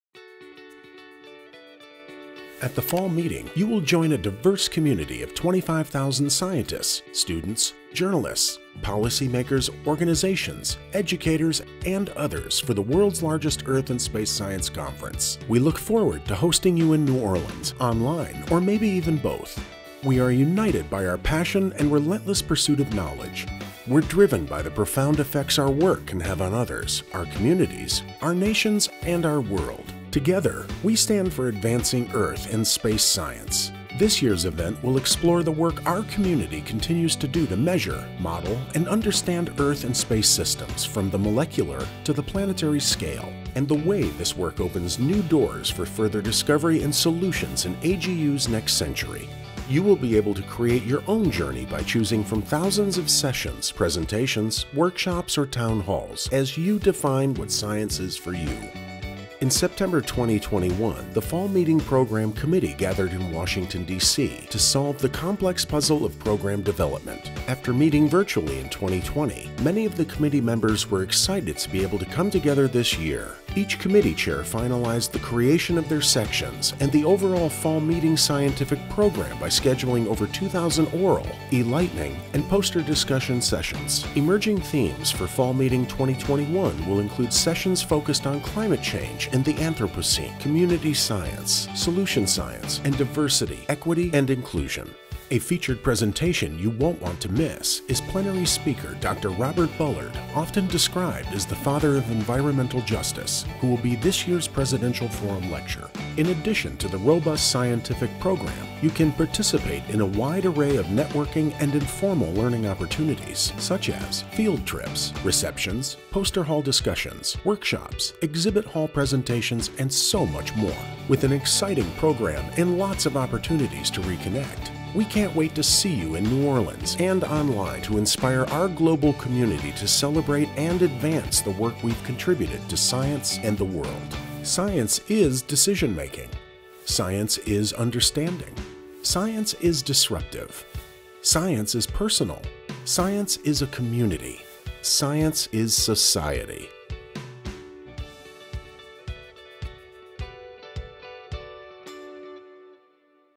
AGU Fall Meeting 2021: We can't wait to see you in New Orleans (or online)!
At AGU Fall Meeting 2021, you will join a diverse community of 25,000 scientists, students, journalists, policymakers, organizations, educators, and others for the world’s largest Earth and space science conference. No matter if you choose to present/speak/attend in-person or online (or a mix of both), AGU21 will provide you numerous opportunities to network with government regulators, scientific visionaries, and industry thought-leaders.

Register and book your housing by 3 November for early bird discount (and to join/renew your AGU membership)

With an exciting program and lots of opportunities to reconnect, we can’t wait to see you in New Orleans and online to inspire our global community to celebrate and advance the work we’ve contributed to science and the world.

Science is decision-making
Science is understanding.
Science is disruptive
Science is personal.
Science is a community.

Science is Society.